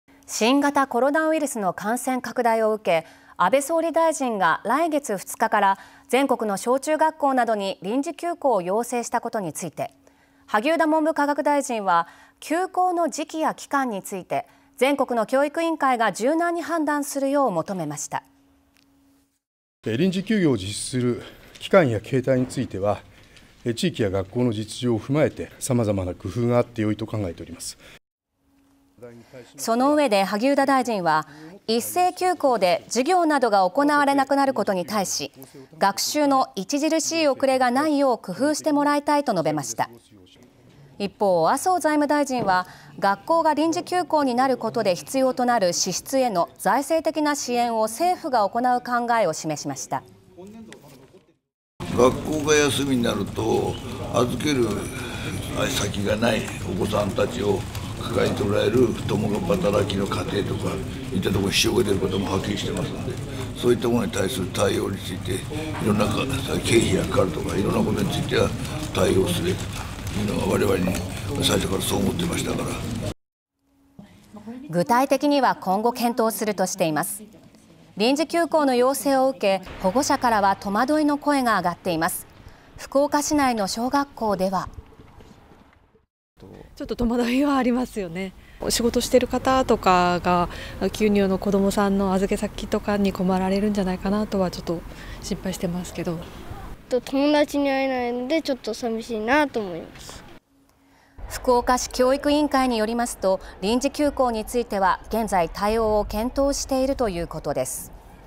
萩生田文科大臣「柔軟に対応を」
新型コロナウイルスの感染拡大を受け、安倍総理大臣が来月２日から全国の小中学校などに臨時休校を要請したことについて萩生田文部科学大臣は、休校の時期や期間について全国の教育委員会が柔軟に判断するよう求めました。
そのうえで、萩生田大臣は一斉休校で授業などが行われなくなることに対し「学習の著しい遅れがないよう工夫してもらいたい」と述べました。

一方、麻生財務大臣は、学校が臨時休校になることで必要となる支出への財政的な支援を政府が行う考えを示しました。具体的には、今後検討するとしています。

臨時休校の要請を受け、保護者からは戸惑いの声があがっています。福岡市教育委員会によりますと、臨時休校については、現在、対応を検討しているということです。